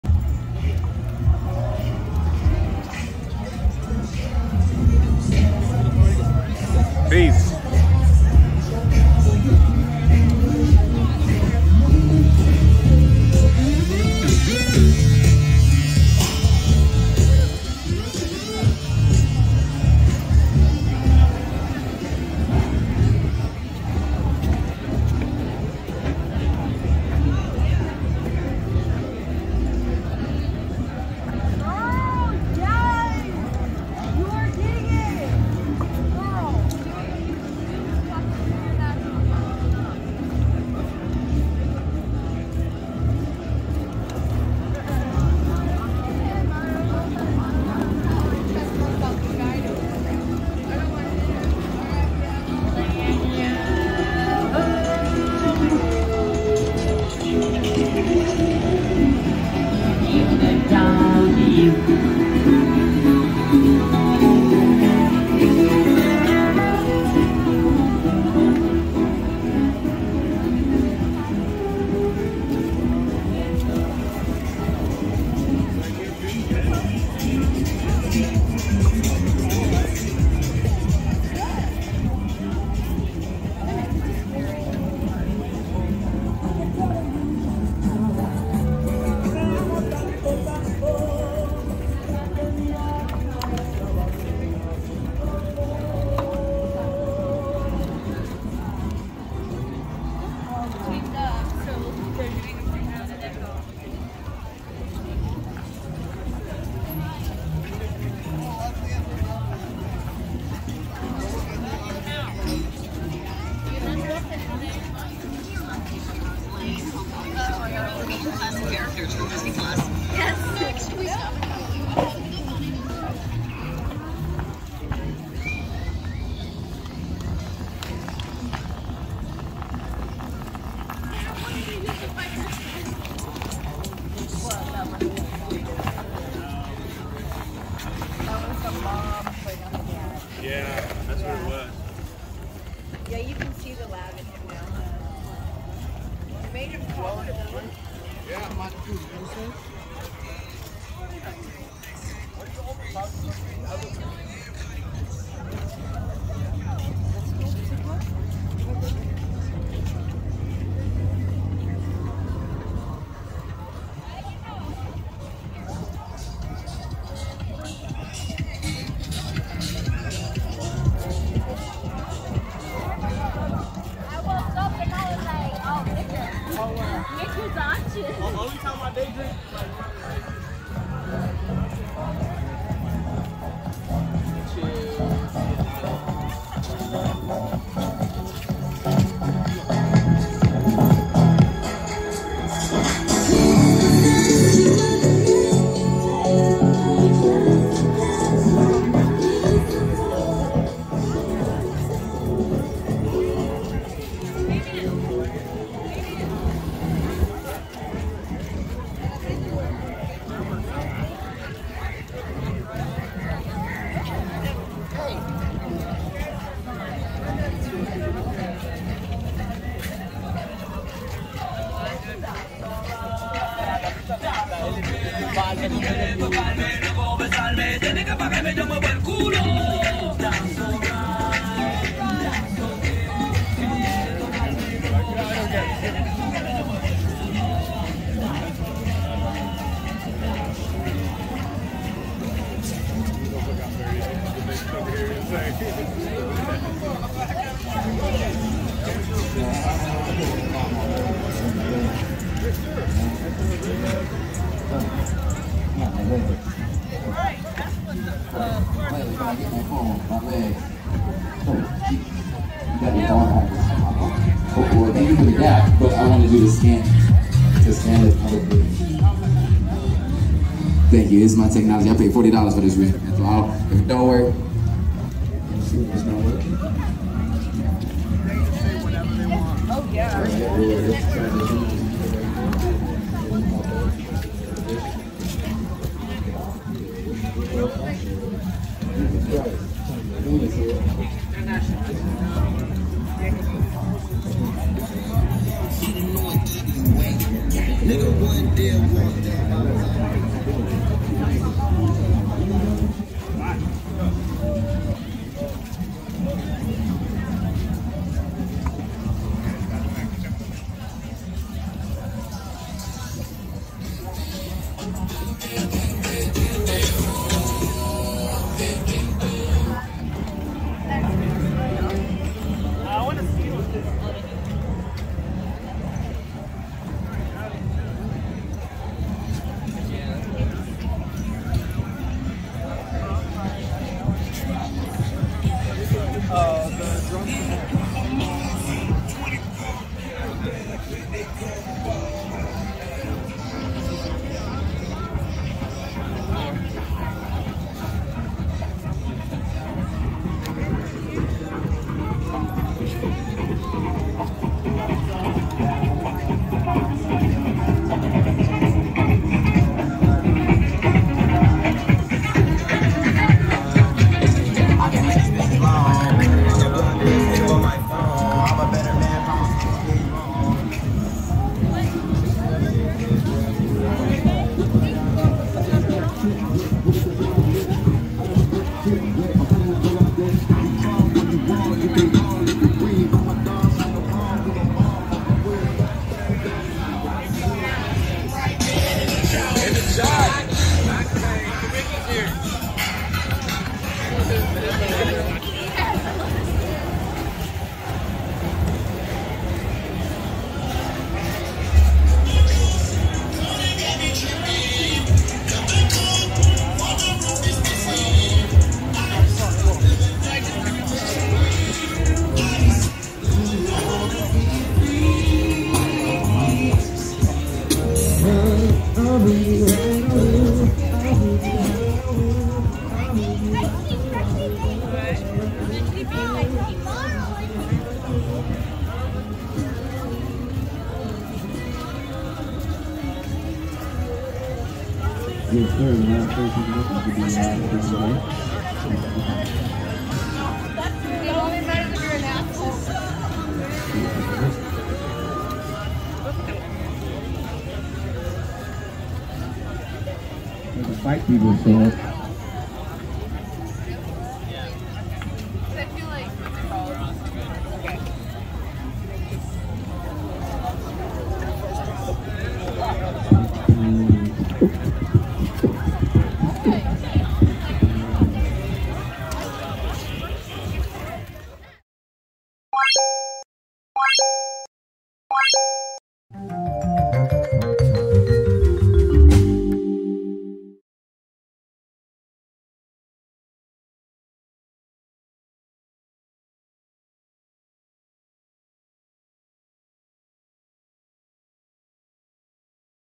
Cruisin' the Venice Boardwalk
A cruise along the Venice Beach Boardwalk on Sunday,  August 14, 2022.

Fabian's photographs have been displayed in museums, art galleries, hospitals, department stores, banks, fire stations, police stations, commercial buildings, amusement parks, offices and homes.